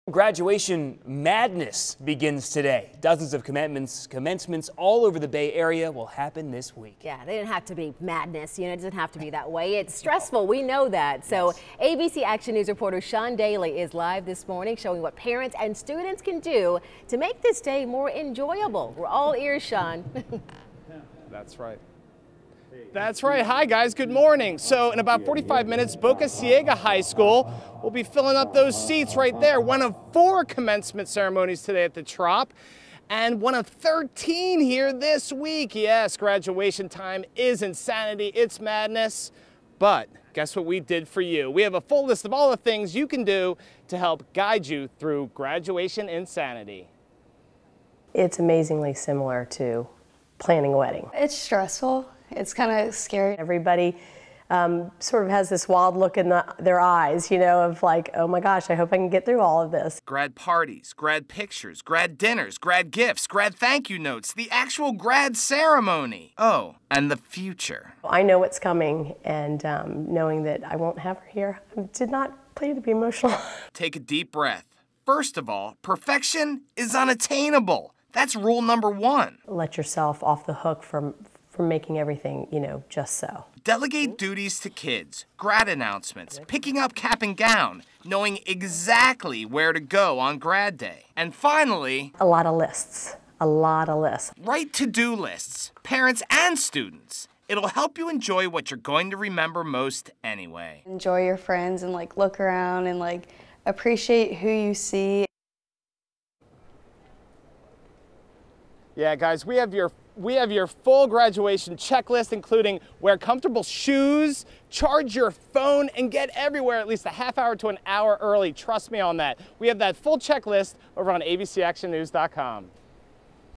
High school graduation stressing you out? How to cope with the end of school madness
High school graduation is an important rite of passage and a maddening whirlwind of parties, gifts, guests, dinners and logistics.